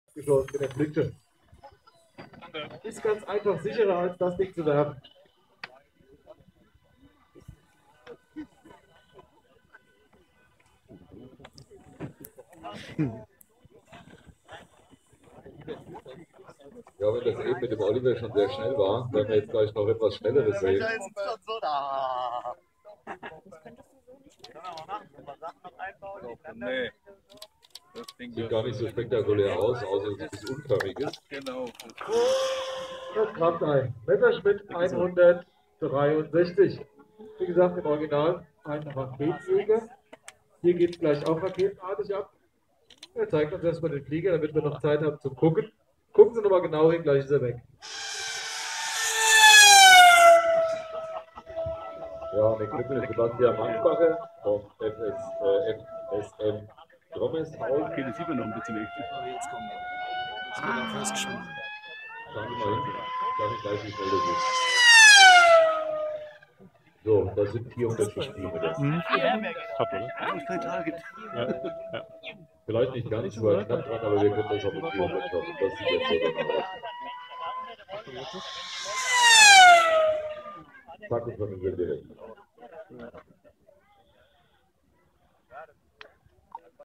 Video vom Flugtag des Kelkheimer Luftsport-Clubs e.V. vom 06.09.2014. Tolles Wetter, toller Tag!